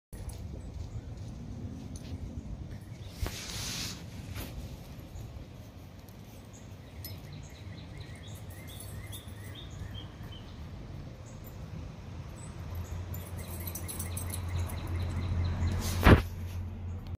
Tim outside down the hill
It was a beautiful day and I wanted to go down the hill to see what the other dogs were doing!